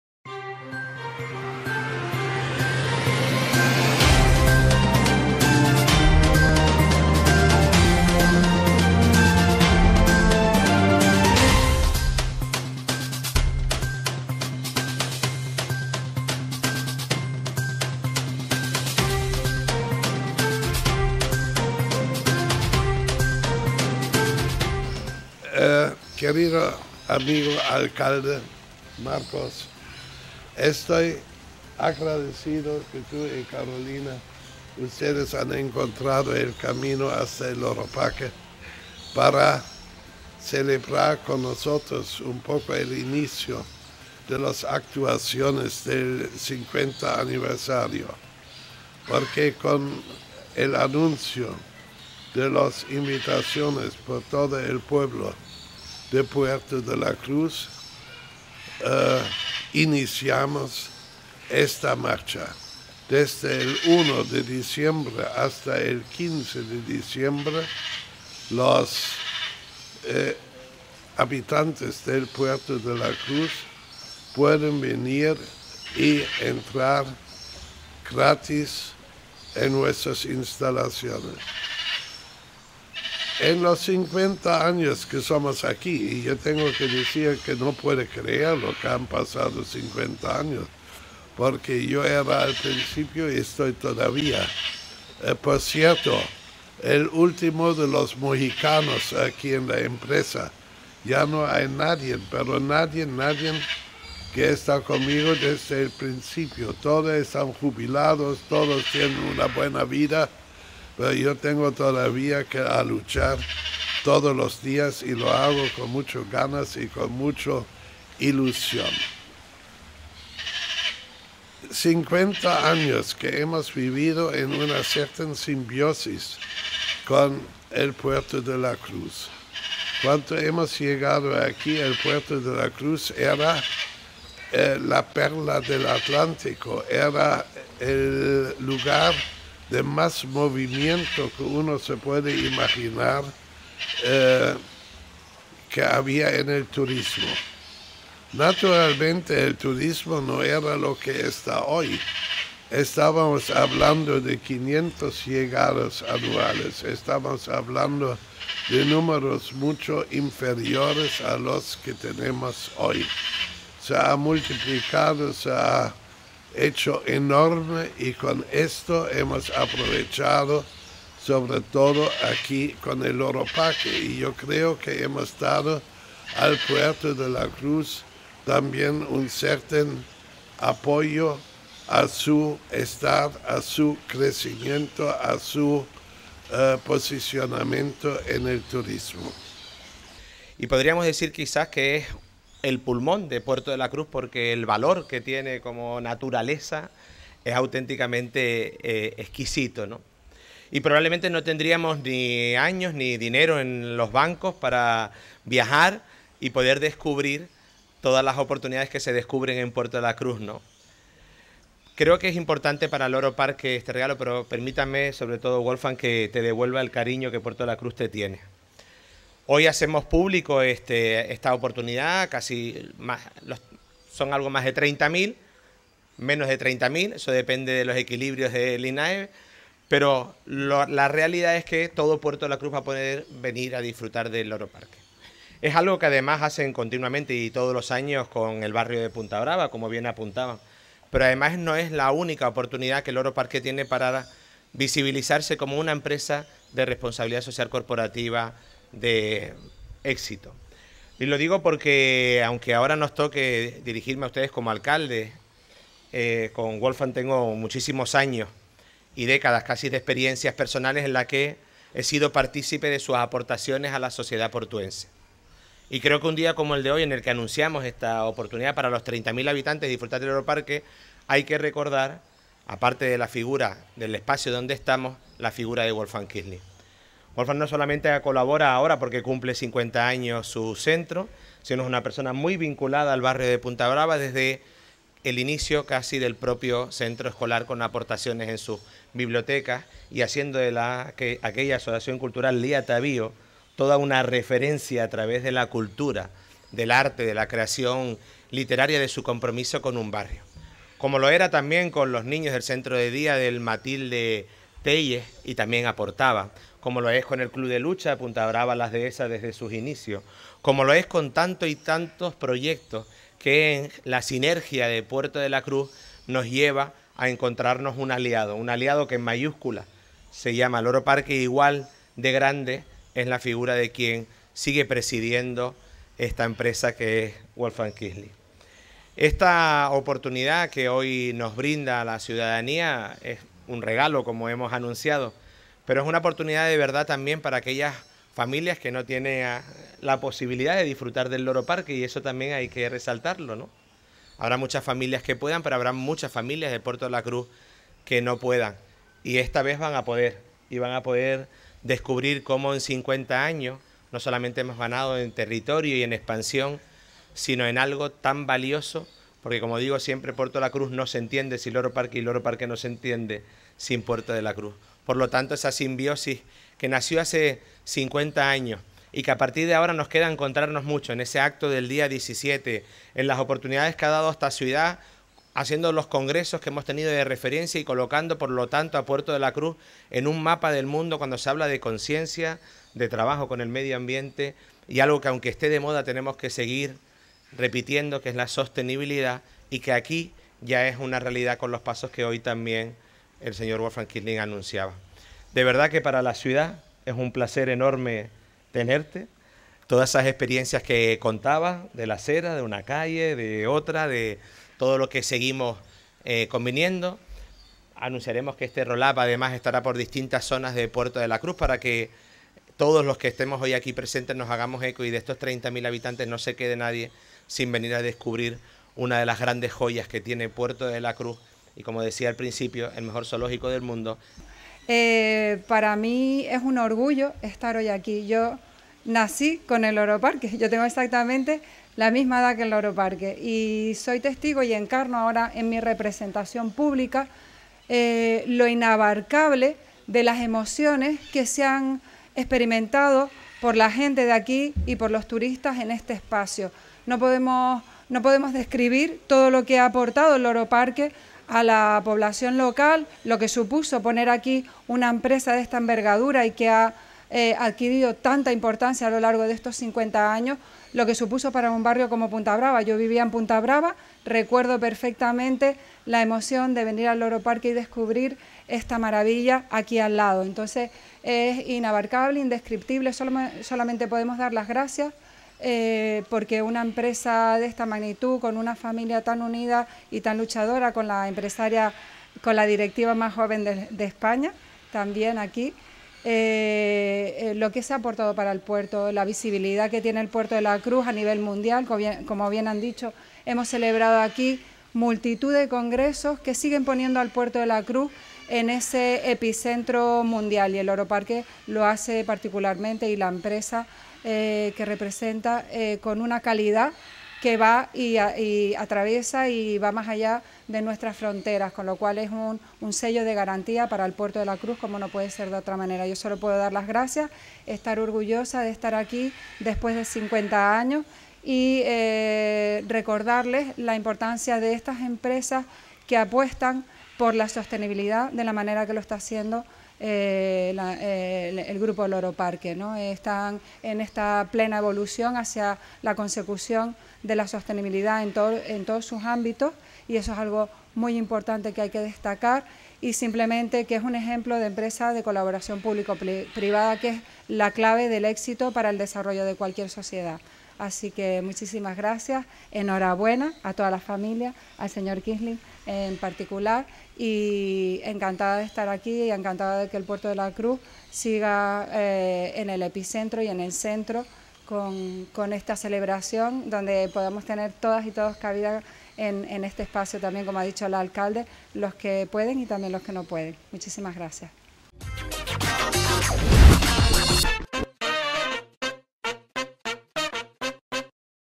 ➖️⏏️ informativos Canal 10📺 | Loro Parque regala 30.000 entradas a los vecinos de Puerto de la Cruz
Loro Parque regala 30.000 entradas a los habitantes del Puerto de la Cruz